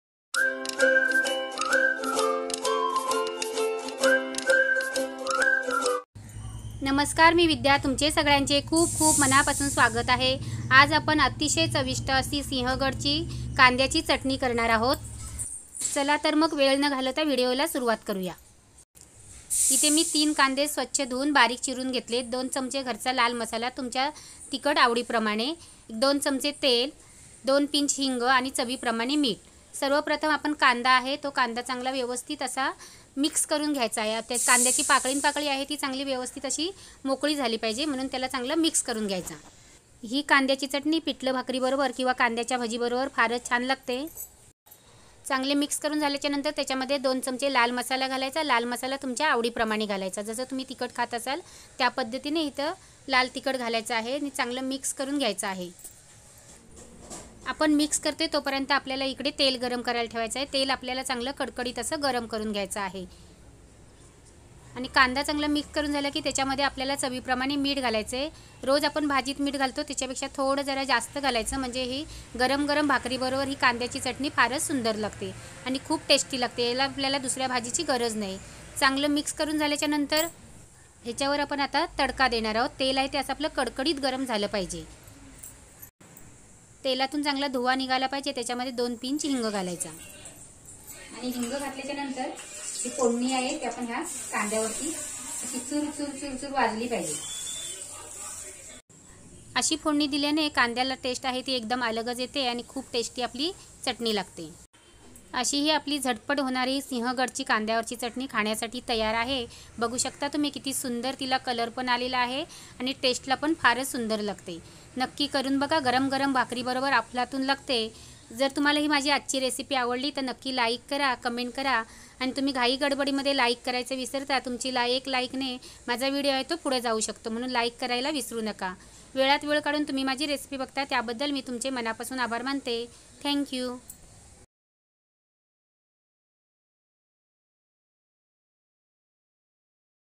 सिंहगडावरील कांद्याची चटणी l Onion Chutney Recipe l chutney Recipe
नमस्कार🙏
खूपच साधी आणि सोपी रेसिपी सिंहगडावरील कांद्याची चटणी
नक्की करून बघा आवडेल तुम्हाला

चटणी काद्याची
चटणी
कांदा भाजी
कांद्याची चटणी
कांद्याची भाजी
सिंहगडावरील चटणी पिठलभाकरी बरोबर खायची चटणी

साहित्य
कांदे 3
लाल मिरची पावडर 2 चमच
हिंग 2 पिंच
मीठ चवीनुसार

ingredients
onion 3
Hing /Asafetida 2 pinch